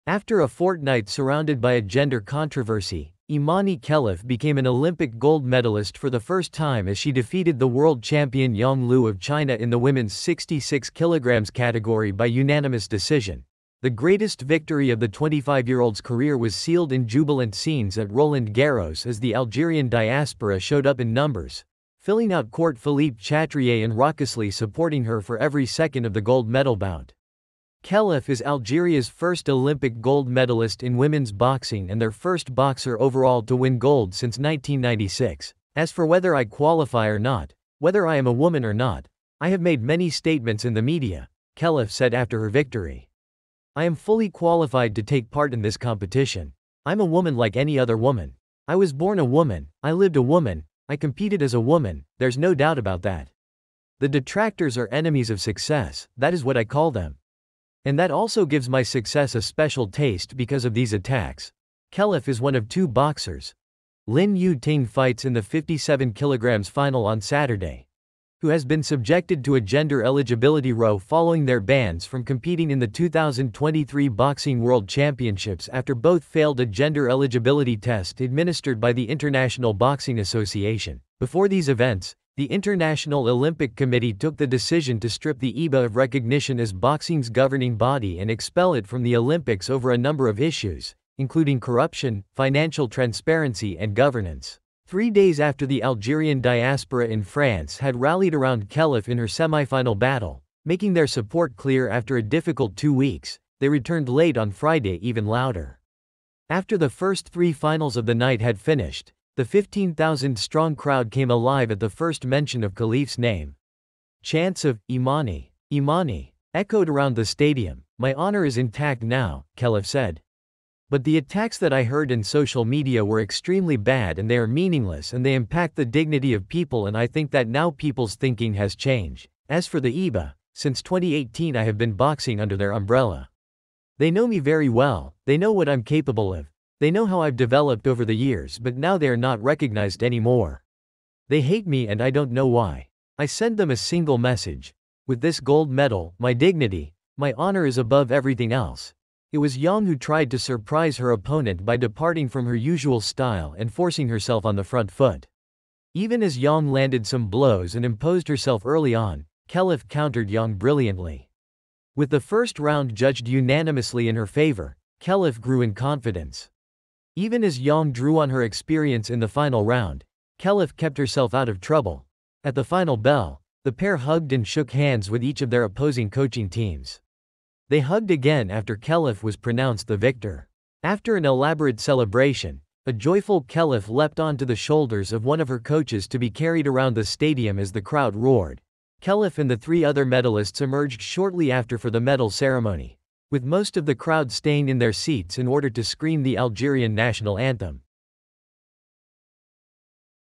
Imane Khelif wins Olympic gold amid gender dispute
The Algerian was banned and reported to have failed gender eligibility tests but, having been allowed to compete in Paris, beat Chinese world champion Yang Liu by unanimous decision over four rounds to win welterweight gold